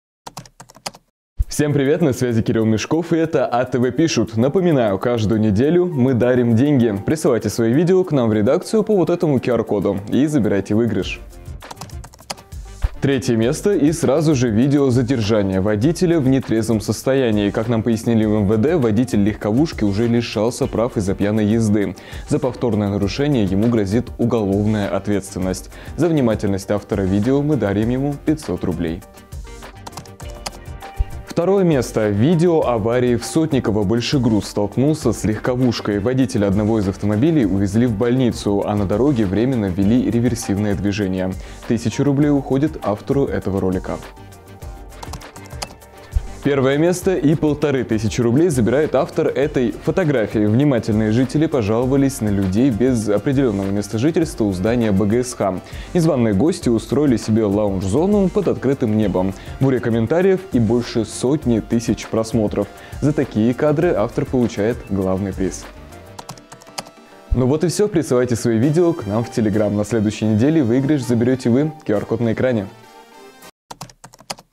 Выиграй 3000 рублей от АТВ! Итоги еженедельного розыгрыша
Как выиграть деньги с помощью своей внимательности? Чтобы принять участие, просто отправьте к нам в редакцию видео с места происшествия и забирайте деньги. Кто выиграл в этот раз - в следующем сюжете.